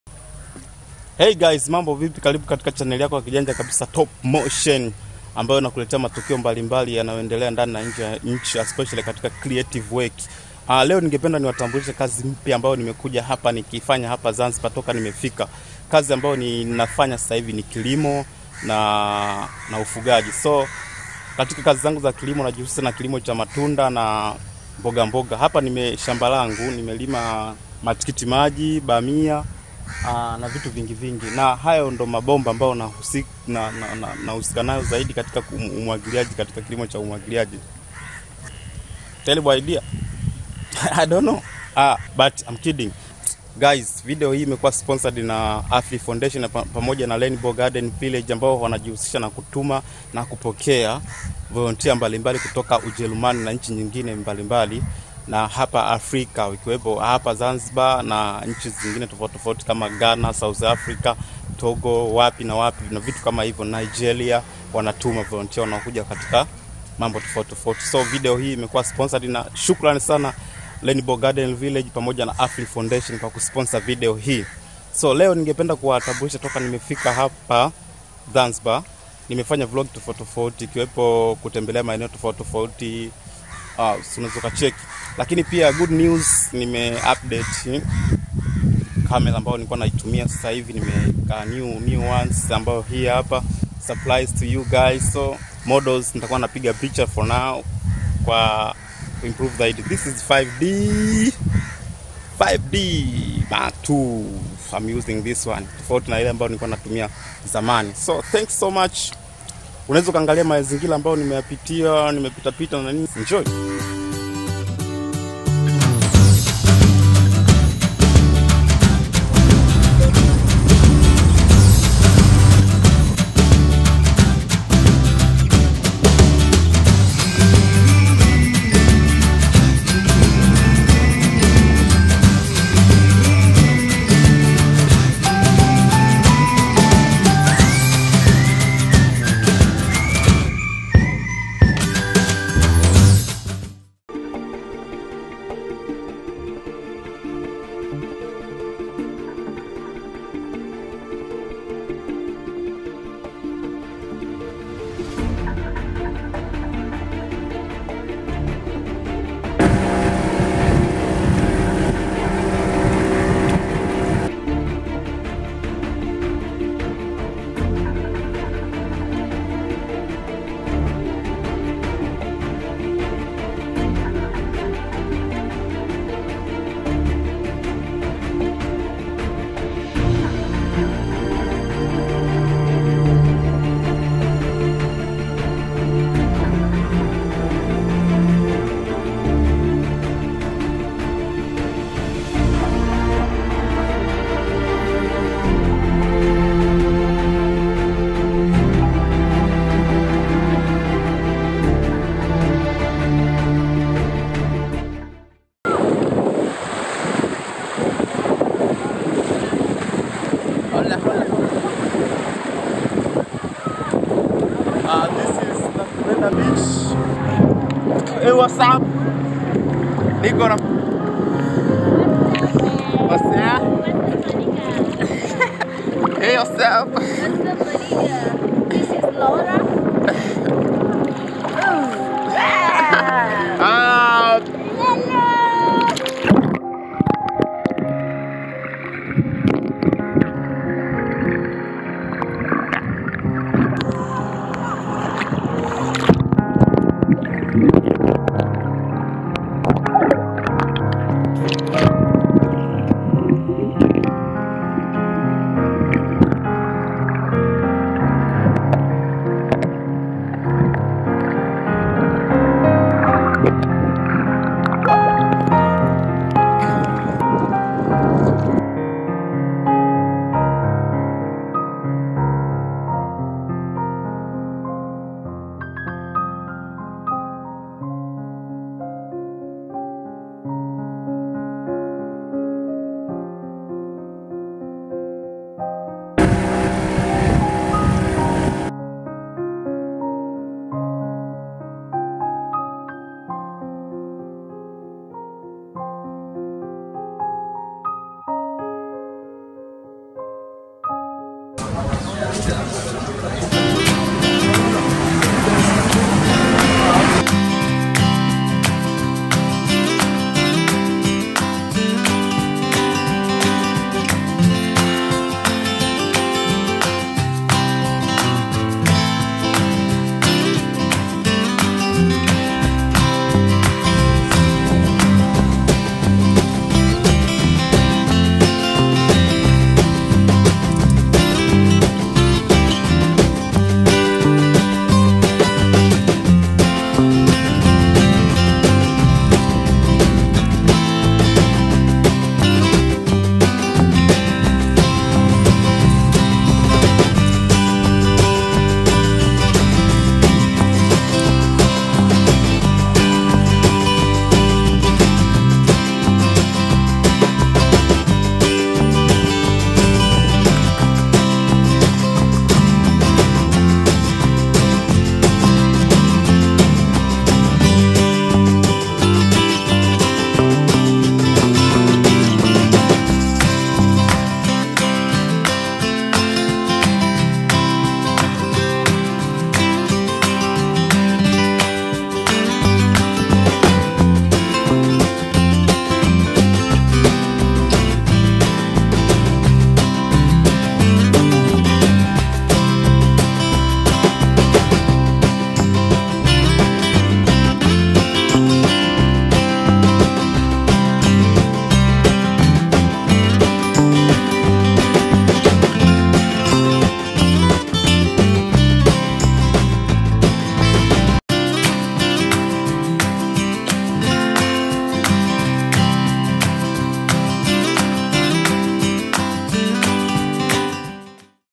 Vivutio vilivyopo zanzibar
This is my first vlog to do in Zanzibar when i visited various attraction areas like national parks,beaches with volunteers from German who are working in various projects here in Zanzibar u  b nder Rainbow garden village and Aflii foundation
Follow me on: